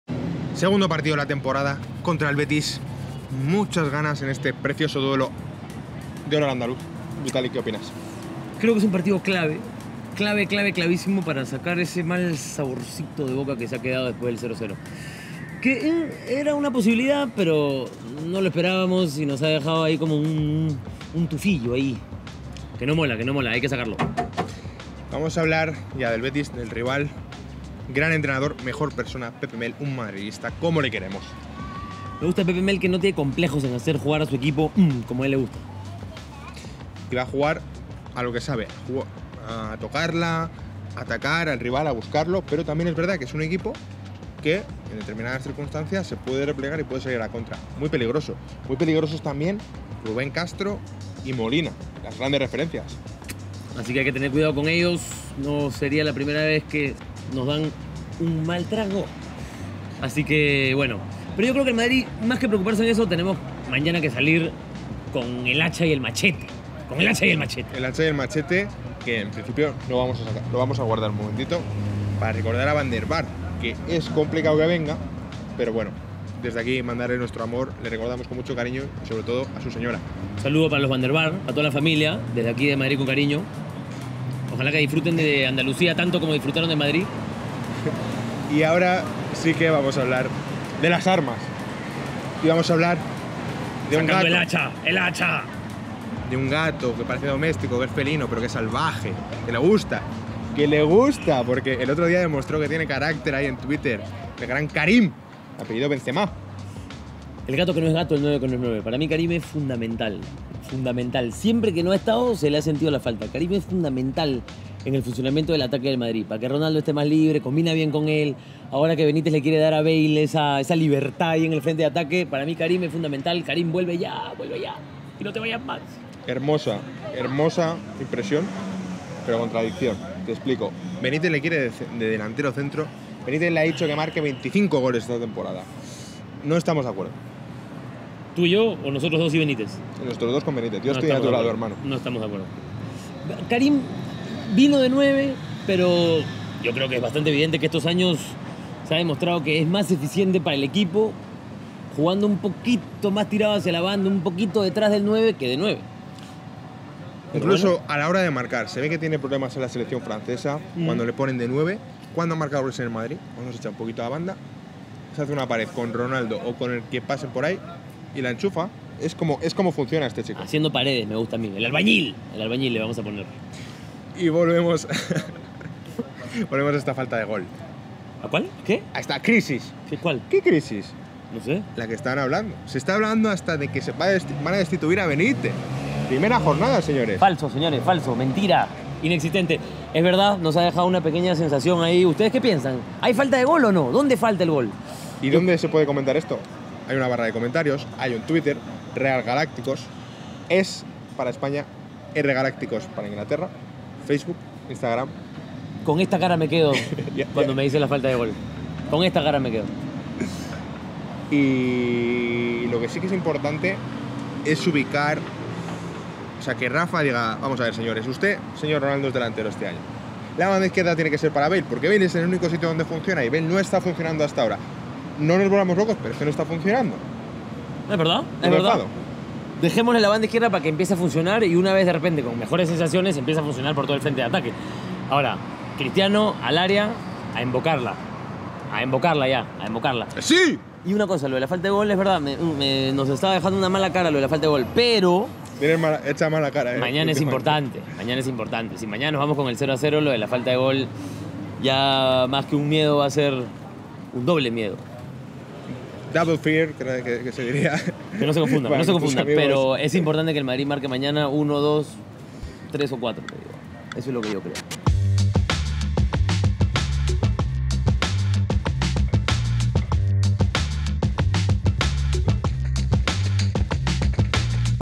Real Madrid vs Betis | Previa Parte 1
Analizamos el encuentro de la Segunda Jornada de La Liga entre el Real Madrid y el Betis. Analizamos al rival y debatimos sobre la crisis del gol y el regreso de Benzema.